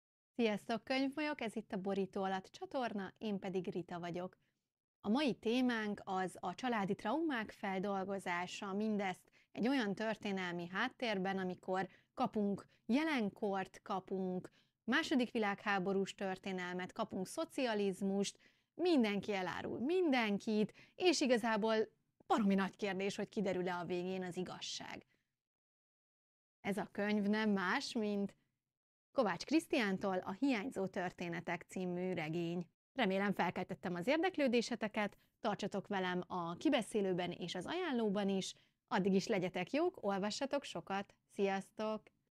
Hiányzó történetek - Kovács Krisztián
Elsőkönyves írótól az ember vagy nagyon sokat vár, vagy pont hogy keveset. Én személy szerint, mivel hallottam már podcast-ben, a Libertine könyvesboltban, és a NIOK Napon is Kovács Krisztiánt beszélni, meg voltam győződve róla, hogy a regénye tele lesz irodalmi utalással, és valami fantasztikusat fog alkotni.

Csatlakozz a többi felülethez is!